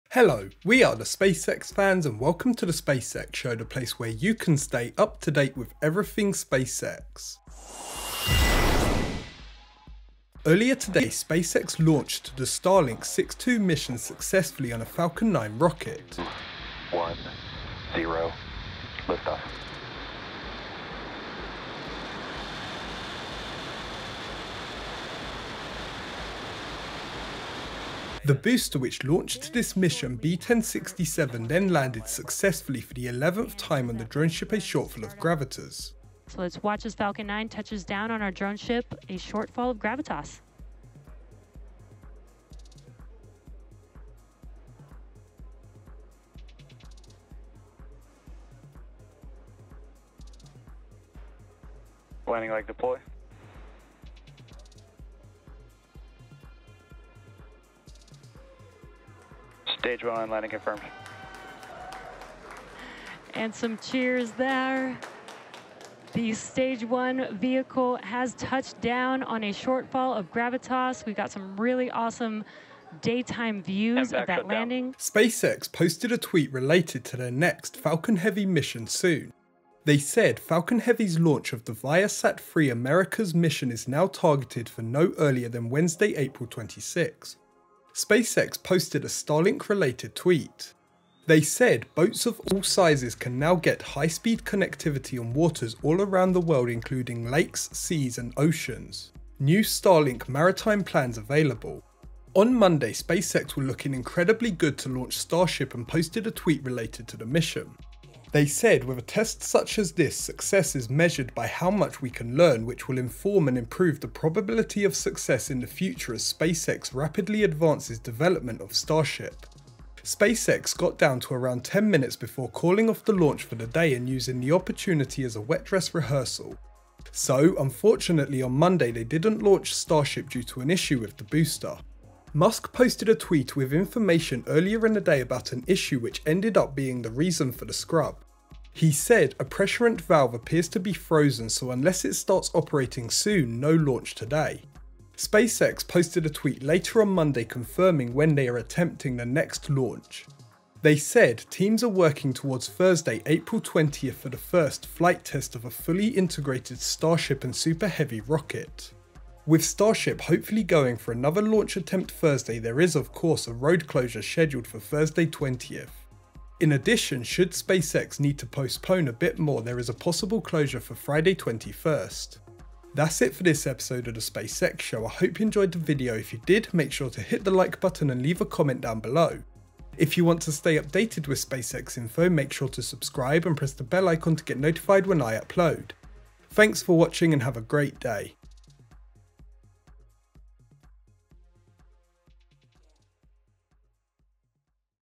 SpaceX Starship Updates! Next Starship Flight Test Attempt Tomorrow! TheSpaceXShow
Welcome to the latest SpaceX Starship updates video from TheSpaceXFans. We have a few updates to start today’s video for the standard SpaceX news. Over in Boca Chica, of course, SpaceX are still looking to launch Starship this week. So, we can hopefully expect to see the next Starship flight test attempt tomorrow! Sit back and enjoy!